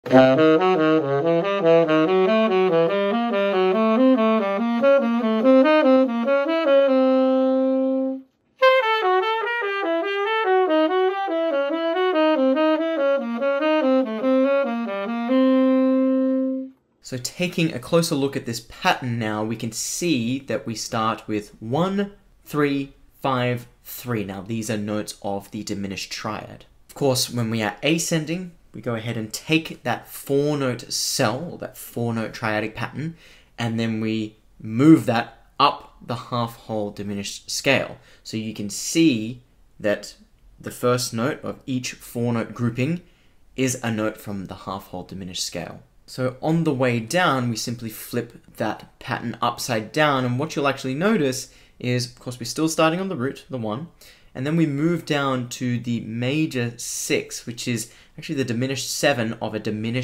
Diminished Triad Exercise For Ultimate Technique
Do you truly have the diminished scales under you fingers? Check out this exercise to help you internalize the Half/Whole Diminished Scale!

One of the Diminished Triad Exercises from our 15 Exercises for Diminished Scale Technique PDF Package!

Exercise Concepts:
Triadic Shapes
Arpeggio Shapes
Diatonic Scale Patterns
Diminished Cell Patterns
Chromatic Approach Notes and Enclosures